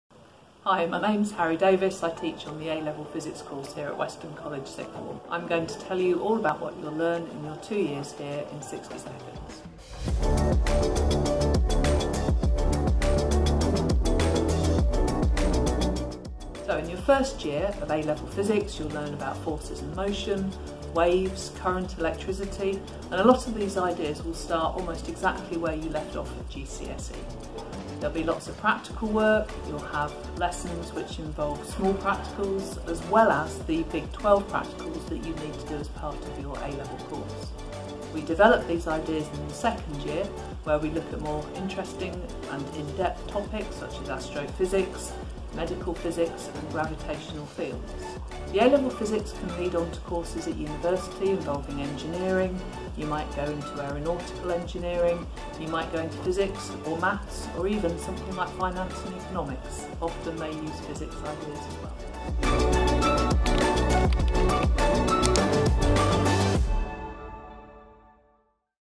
A Level Physics in 60 Seconds | Weston College Sixth Form
What do you get up to in A Level Physics?? Here's all you need to know in just 60 seconds!